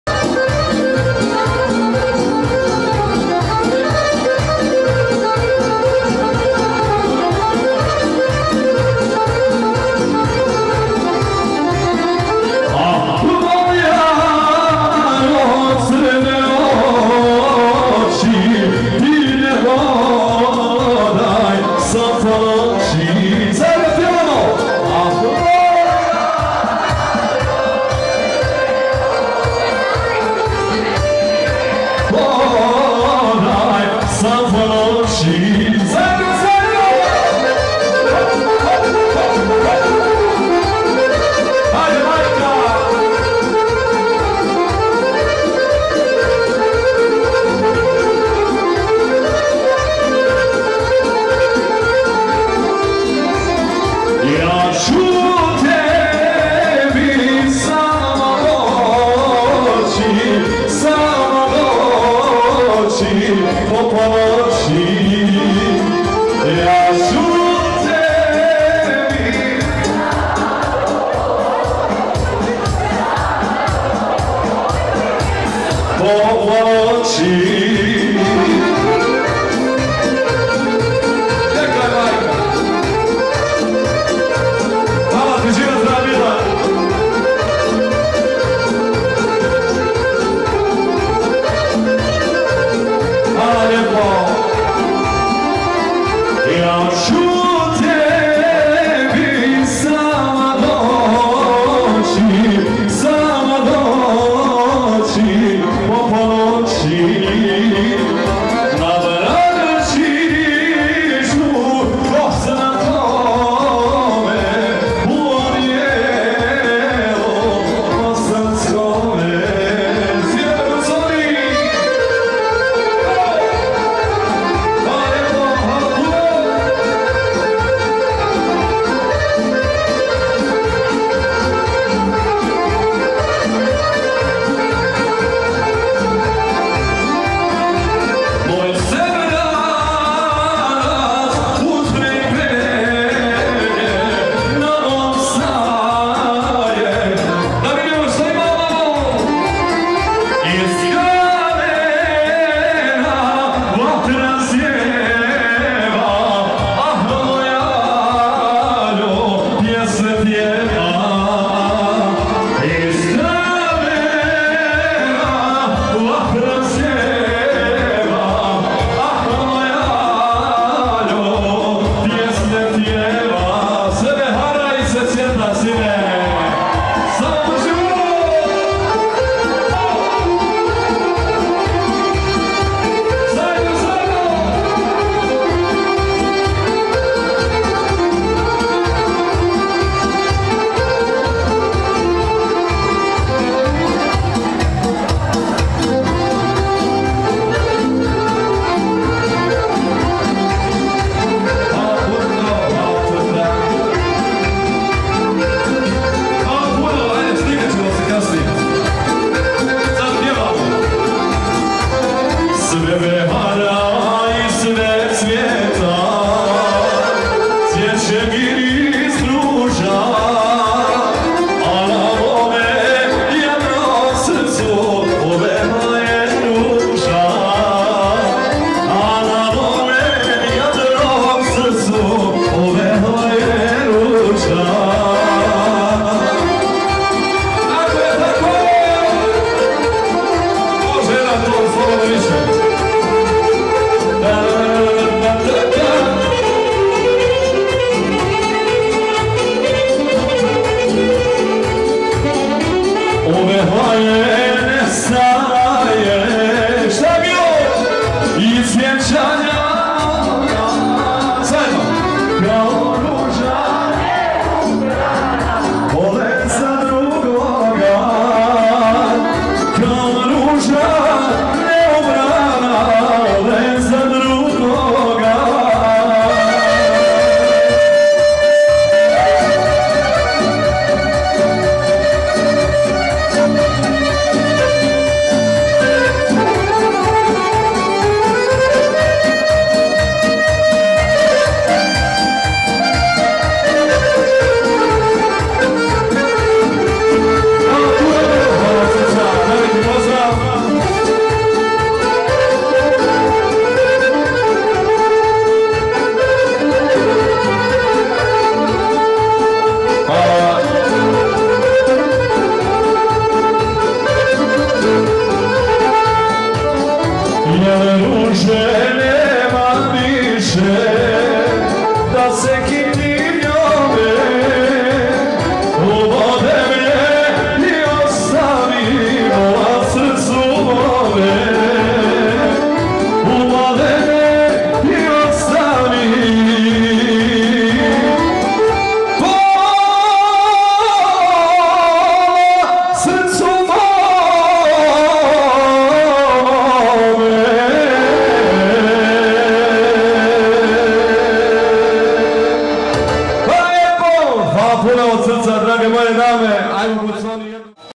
Mirza Šut ~ sevdalinke - 1300 žena zabava FIS Vitez 12.11.2022.
Svadbeni salon FIS Vitez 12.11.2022.
Label and copyright : Autor snimatelj Ibro Zahirovic